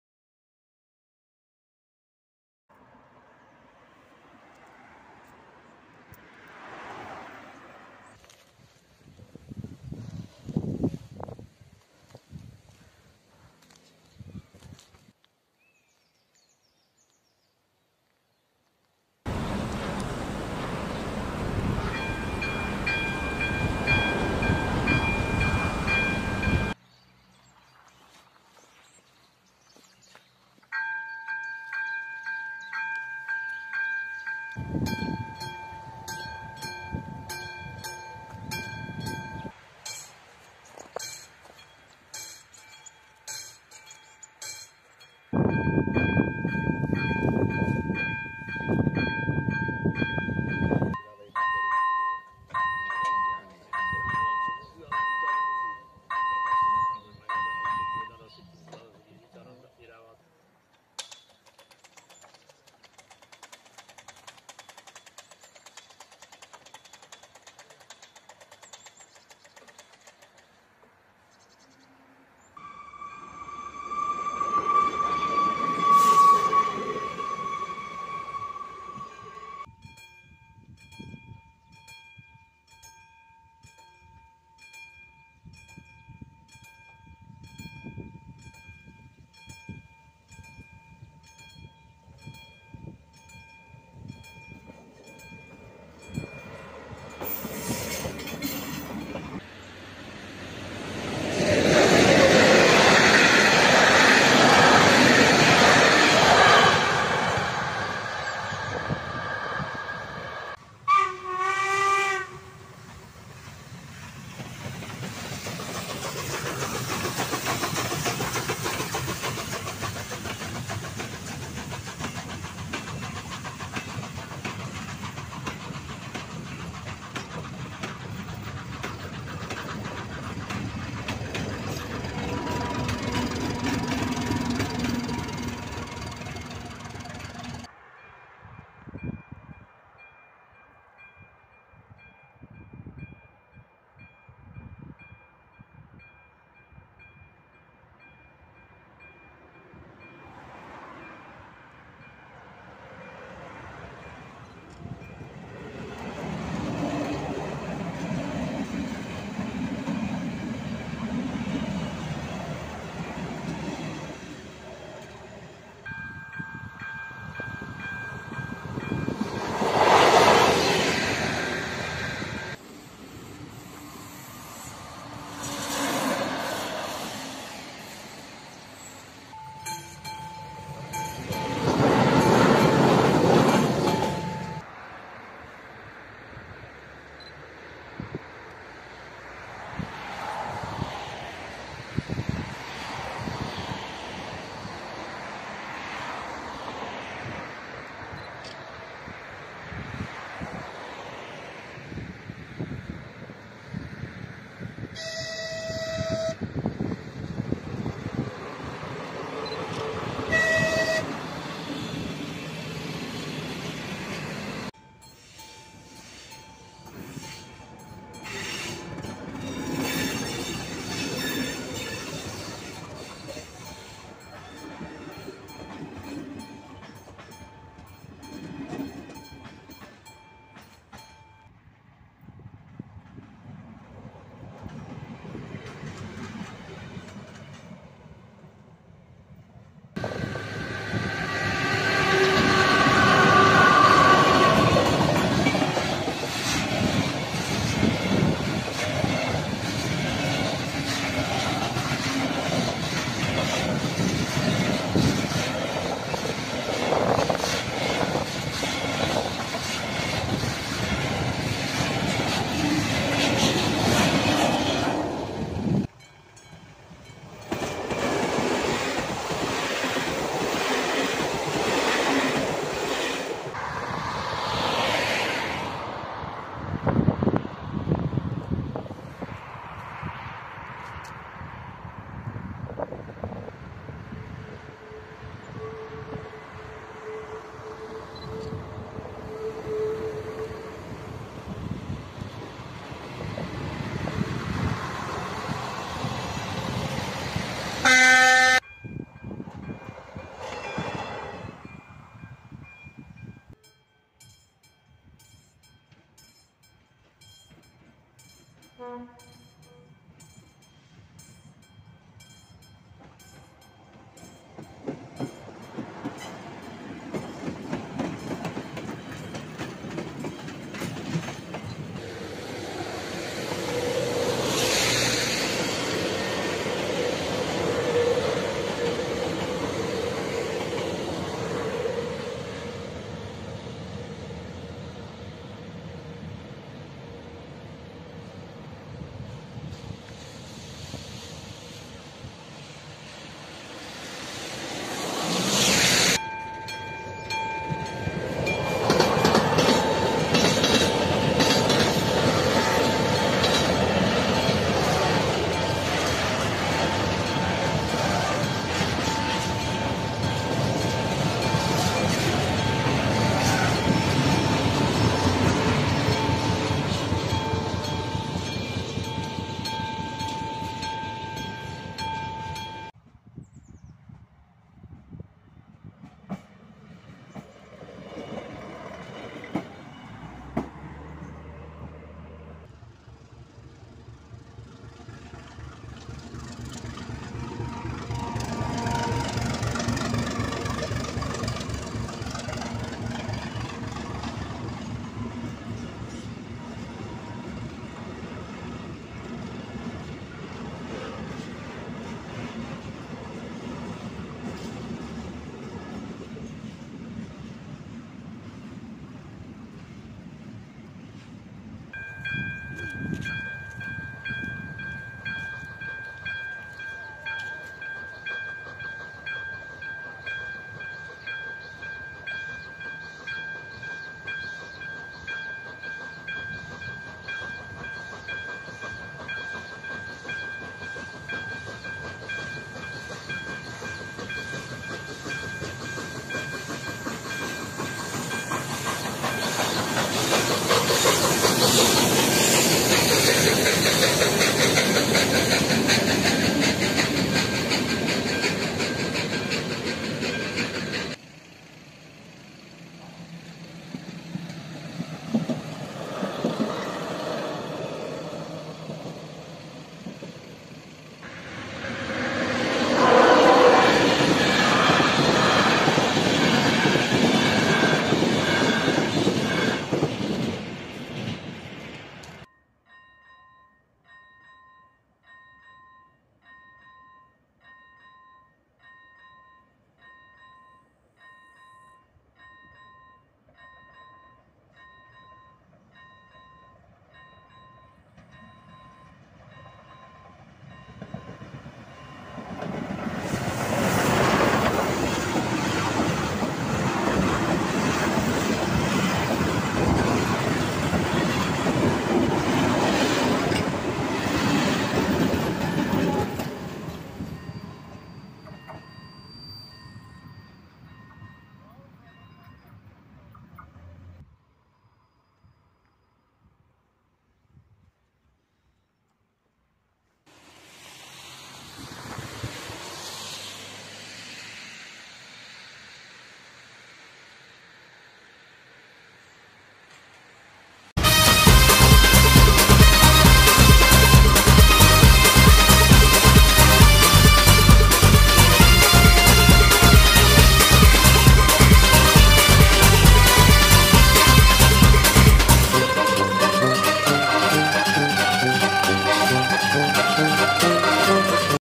Ewald Herink-level crossing - Czech level crossing 2021
Přejezdový sestřih natočených přejezdů za rok 2021